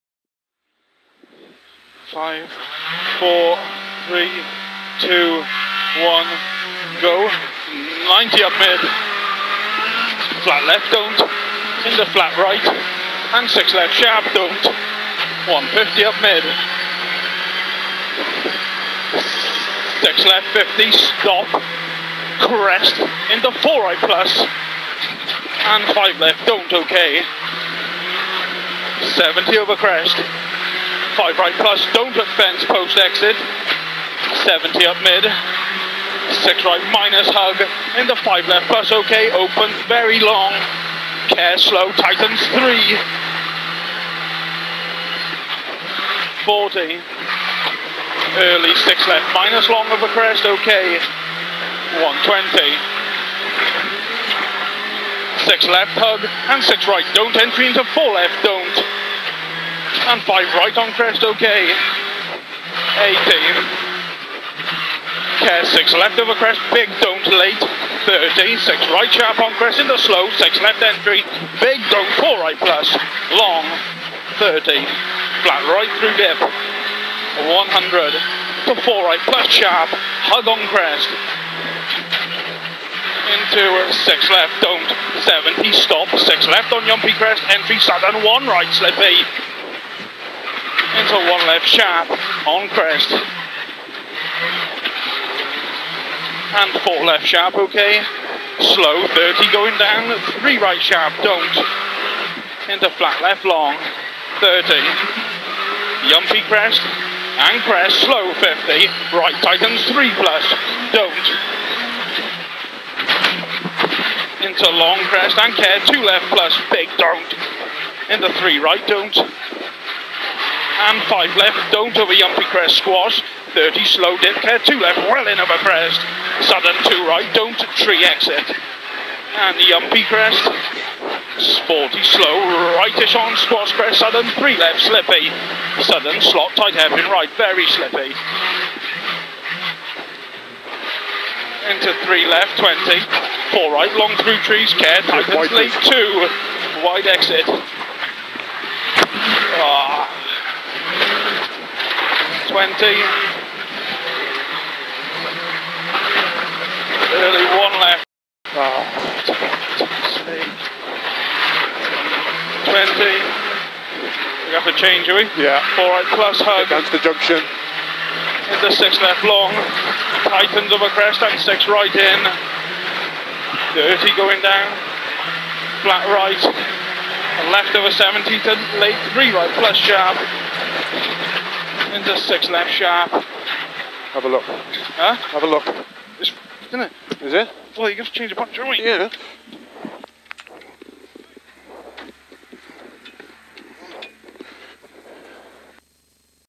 CAMBRIAN RALLY 2020 / TOM CAVE / ONBOARD / SS3 Penmachno
The margins are small, we clipped a rock with the rear left wheel, and broke the wheel and the brake disc. Forced to stop and change wheel, we lost over 3 minutes, and with it the rally lead.

British Rally Championship / round 1 / Cambrian Rally 2020
Follow me on...

SS6 PENMACHNO / / Dale Furniss calling the Pacenotes

Phillip Case Rally Sport prepared Hyundai I20 R5 / Pirellli Tyres / Shukers Hyundai Aberystwyth.